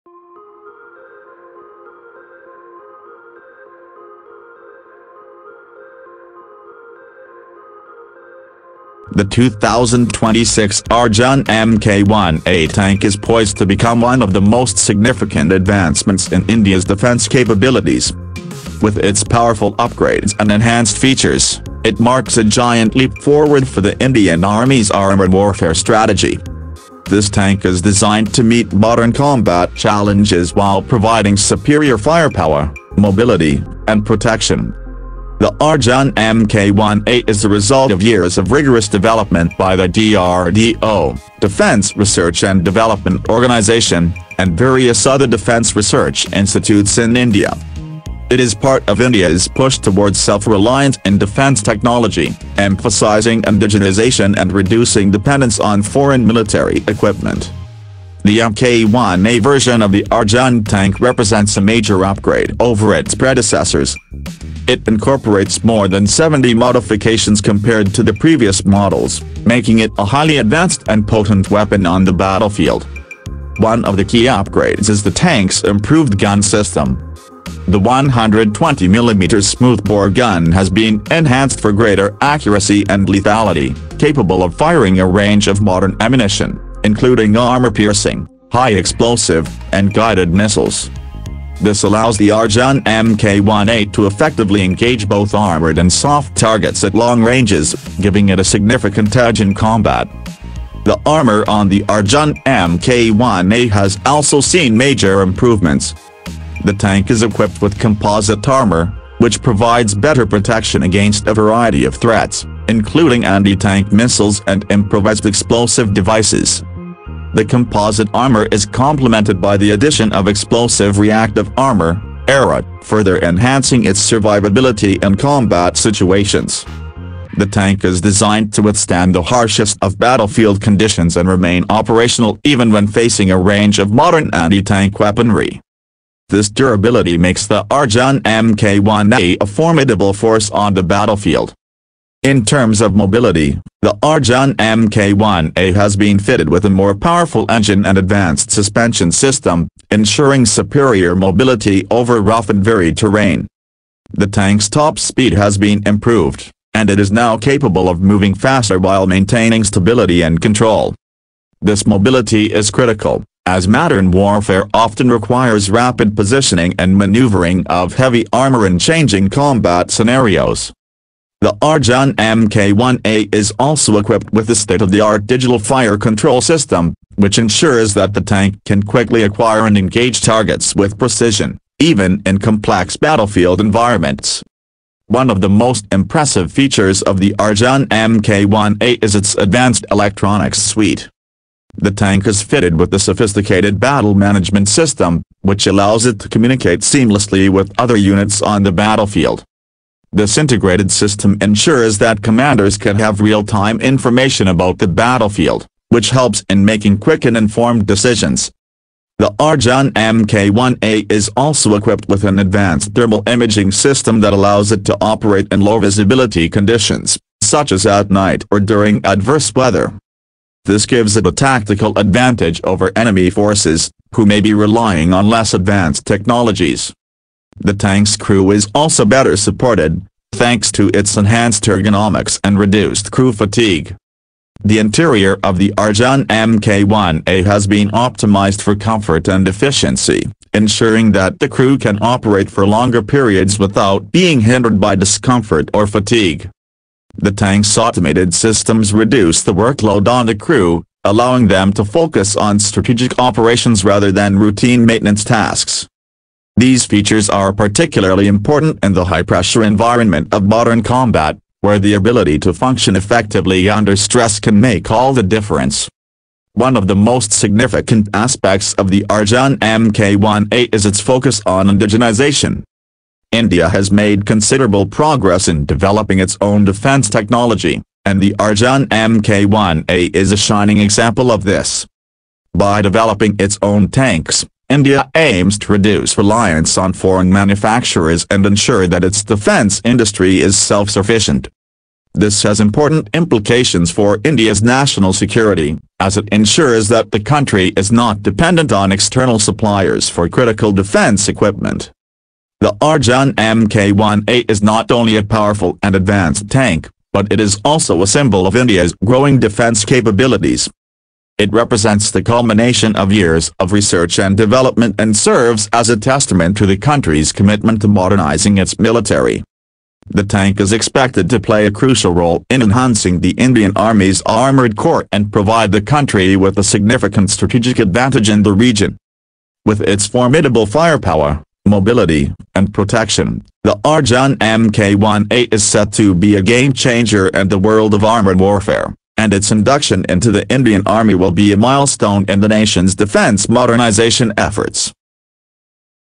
Arjun Mk1A 2026 Update: India's Most Advanced Battle Tank Unveiled
The 2026 Arjun Mk1A tank is poised to become one of the most significant advancements in India’s defense capabilities. With its powerful upgrades and enhanced features, it marks a giant leap forward for the Indian Army’s armored warfare strategy. This tank is designed to meet modern combat challenges while providing superior firepower, mobility, and protection. The Arjun Mk1A is the result of years of rigorous development by the DRDO (Defence Research and Development Organisation) and various other defense research institutes in India. It is part of India’s push towards self-reliance in defense technology, emphasizing indigenization and reducing dependence on foreign military equipment.

The Mk1A version of the Arjun tank represents a major upgrade over its predecessors. It incorporates more than 70 modifications compared to the previous models, making it a highly advanced and potent weapon on the battlefield. One of the key upgrades is the tank's improved gun system. The 120mm smoothbore gun has been enhanced for greater accuracy and lethality, capable of firing a range of modern ammunition, including armor-piercing, high-explosive, and guided missiles. This allows the Arjun Mk1A to effectively engage both armored and soft targets at long ranges, giving it a significant edge in combat